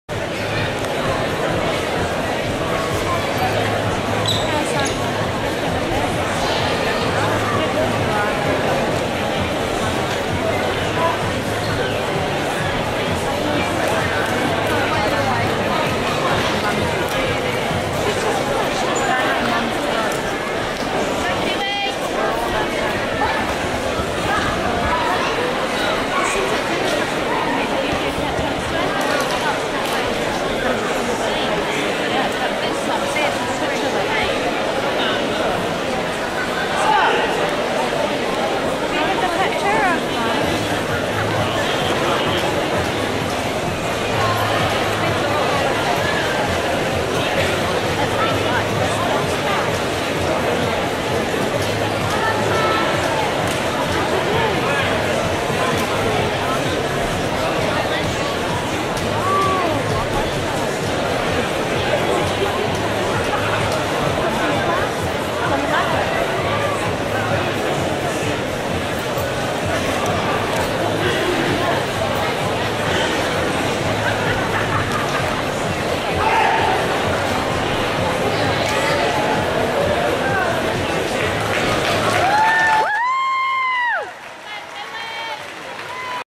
gkr wc5 16 08 2009 sun ring3 snr male team kata 18+ 08b bassai dai
GKR Karate
World Cup 5
Melbourne, Australia
State Netball Hockey Centre
10 Brens Drive, Parkville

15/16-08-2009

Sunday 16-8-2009

Arena1 Ring3 Senior Male Team Kata (18yrs+)